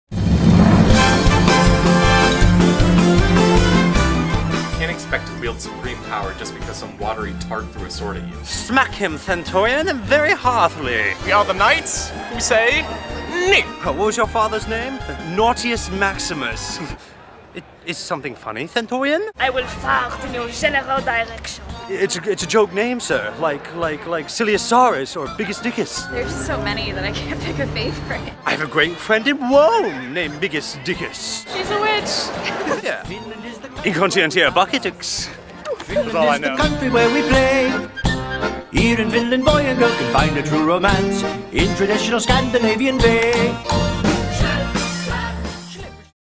What's your favorite Spamalot quote?
Monty Python's Spamalot
The School of Theatre at Florida State University
February 14 - March 2, 2014
The Fallon Theatre
Directed by Alan Souza